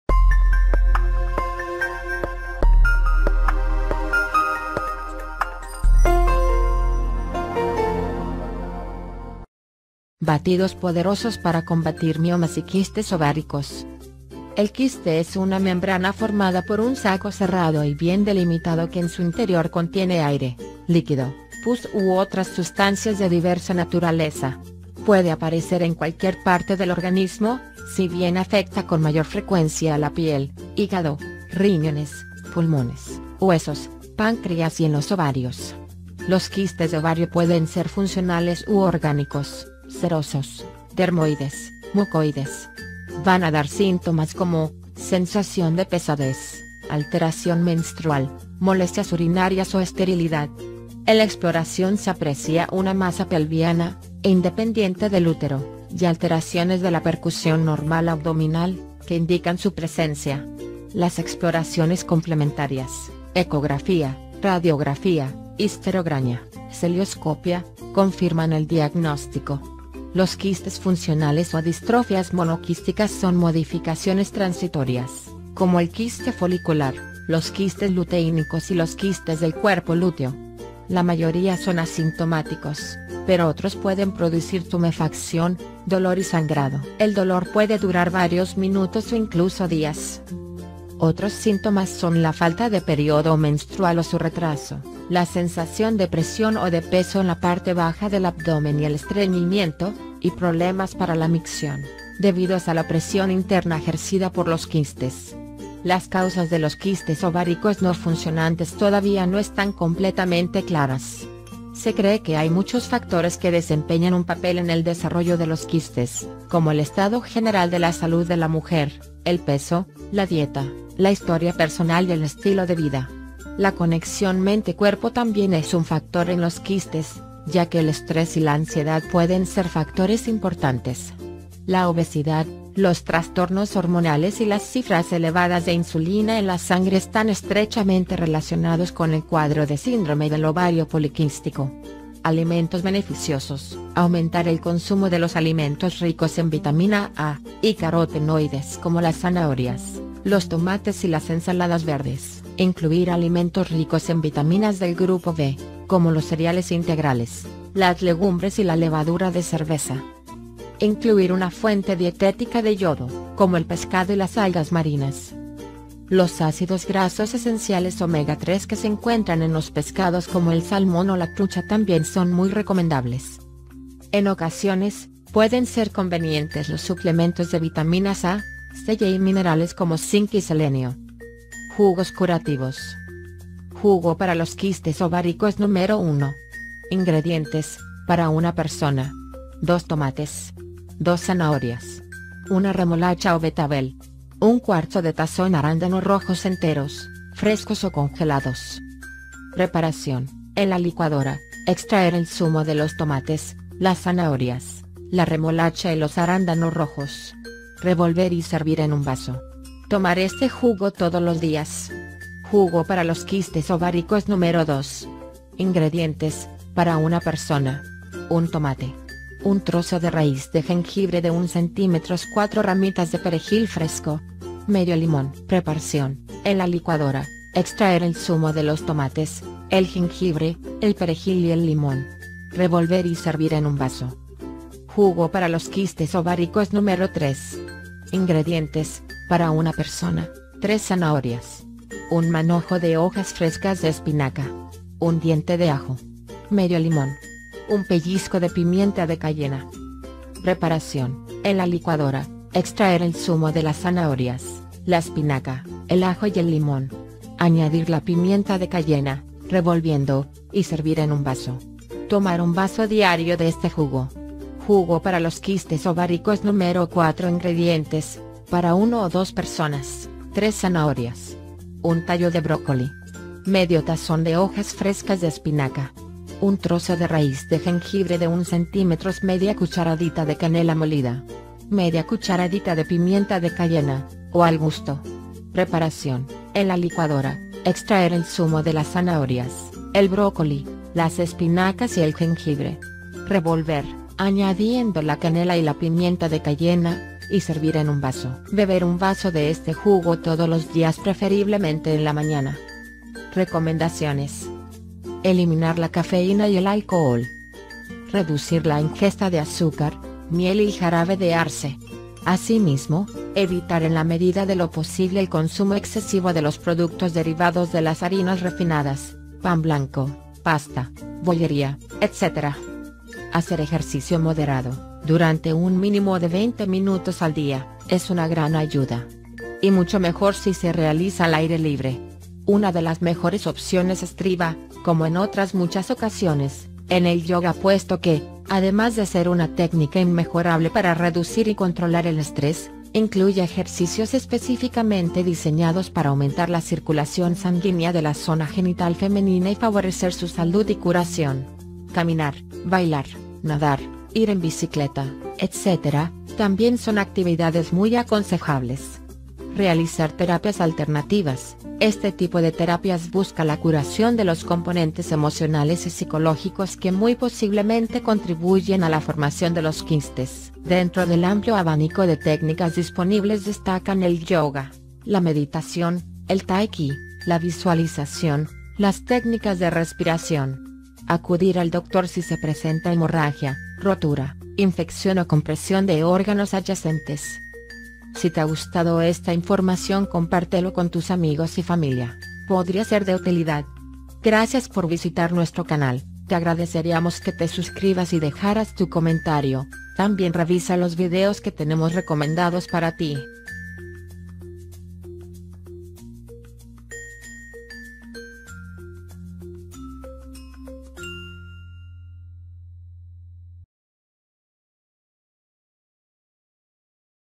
BATIDOS Para Combatir Miomas Y Quistes Ováricos De Manera Natural
Batidos poderosos para combatir miomas y quistes ováricos de manera con este remedio casero natural en la que te podrás sentir aliviado de estos molestosos y odiosos quistes y miomas que toda mujer puede llegar a sufrir.

Existen formas para poder combatir este mal, lo primero es consumir buenos alimentos, así es, aumentar el consumo de los alimentos ricos en vitamina A, y carote-noides como la zanahoria, los tomates y las ensaladas verdes. Incluir alimentos ricos en vitaminas del grupo B, como lo pueden ser los cereales integrales, las legumbres y la levadura de cerveza, también una fuente dietética de yodo, como el pescado y las algas marinas.

Los ácidos grasos esenciales omega 3 se logran encontrar en los pescados como el salmón o la trucha, son muy recomendables. En ocasiones pueden ser convenientes los suplementos de vitaminas A, C y E, junto con los minerales como el Zinc y Selenio.

Para los jugos curativos, el poder encontrar los ingredientes será cosa fácil, no habrá que hacer grandes y largas compras en tiendas. Puedes preparar tú mismo, unos excelentes jugos para combatir los quistes ováricos.

Ahora, para la preparación.

Para el primero, los ingredientes serán (en caso de que sea para una sola persona) 2 tomates, 2 zanahorias, 1 remolacha o betabel, cuarto de tazón de arándanos rojos enteros, frescos o congelados.

En la licuadora se deberá de extraer el zumo de los tomates, zanahorias, la remolacha y los arándanos rojos, luego revolver y servir un vaso. Se debe de tomar este jugo todos los días para poder observar con gran eficacia los resultados.

Para el jugo curativo número 2, los ingredientes serán un tomate, 1 trozo de raíz de jengibre de un cm, 4 ramitas de perejil fresco y ½ limón. Su preparación se hará en la licuadora, se extraerá el zumo de los tomates, el jengibre, el perejil y el limón. Finalmente se deberá de revolver y servir en un vaso, al igual que el otro jugo, se debe tomarlo todos los días para resultados.

El jugo para los quistes ováricos número 3, se hará con los ingredientes de 3 zanahorias, 1 manojo de hojas frescas de espinaca, 1 diente de ajo, ½ limón y 1 pellizco de pimienta de cayena, en la preparación se hace nuevamente en la licuadora, se extrae el zumo de las zanahorias, la espinaca, el ajo y el limón, se añade la pimienta de cayena, se revuelve y se sirve finalmente en un vaso.

Para que todo lo anteriormente mencionado pueda surgir efecto, se debe de tomar el remedio casero todos los días, solo de esta forma se podrán ver los resultados que se desean, y podrá corroborar que los remedios naturales son suficientes para cualquier ocasión.